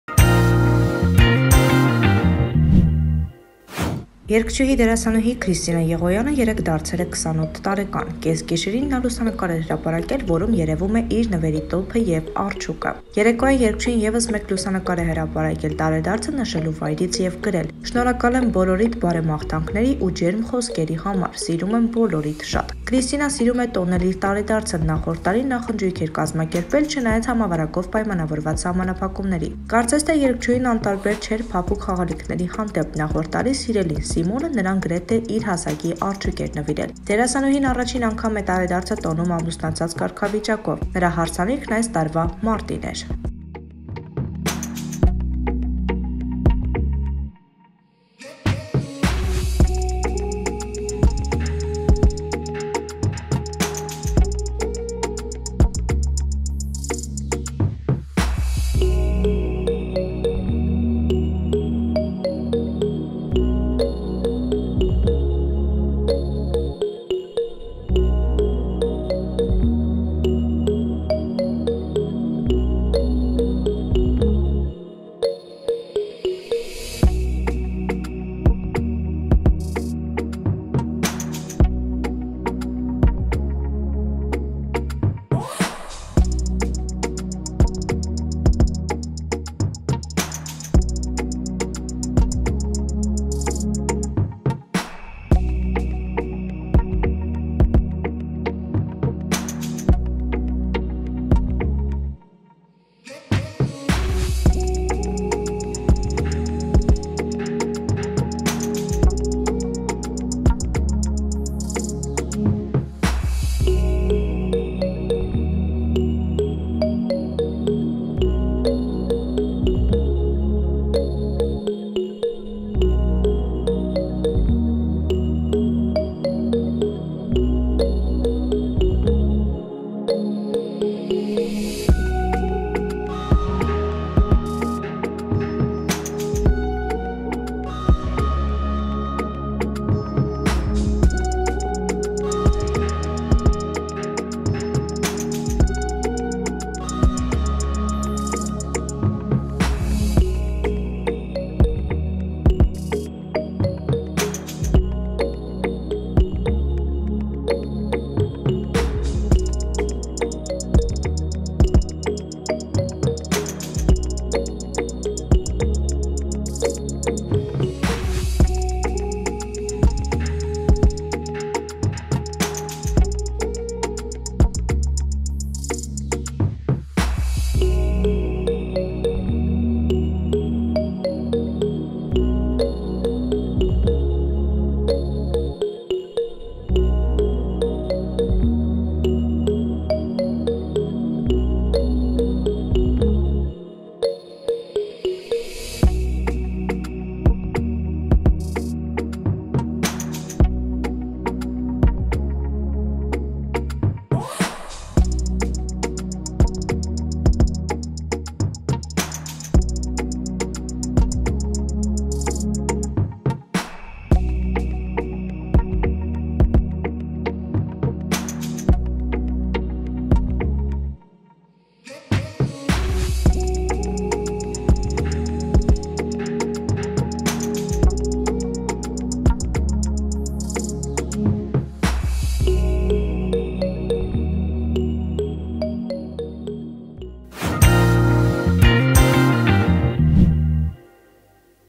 Քրիստինա Եղոյանի 28-ամյակը
Դերասանուհին առաջին անգամ է տարեդարձը տոնում ամուսնացած կարգավիճակով, նրա հարսանիքն այս տարվա մարտին էր։

Մանրամասն՝ տեսանյութում